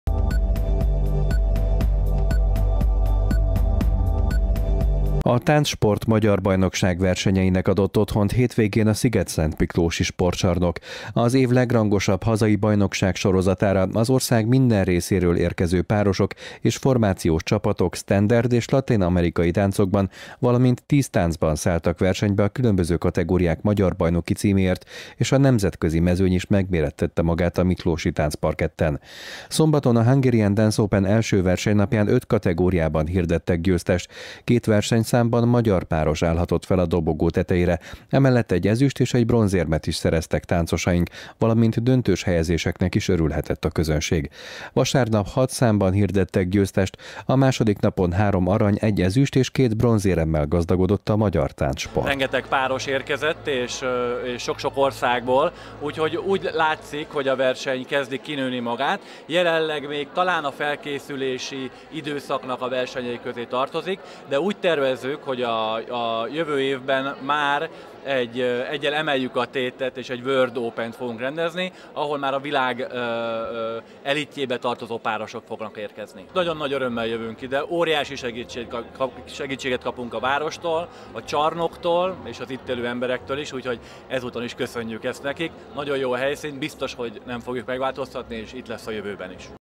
A TáncSport Magyar Bajnokság versenyeinek adott otthont a szigetszentmiklósi sportcsarnok
Az év legrangosabb hazai bajnokságsorozatára az ország minden részéről érkező párosok és formációs csapatok standard és latin-amerikai táncokban, valamint tíztáncban szálltak versenybe a különböző kategóriák magyar bajnoki címéért és a nemzetközi mezőny is megmérettette magán a miklósi táncparketten.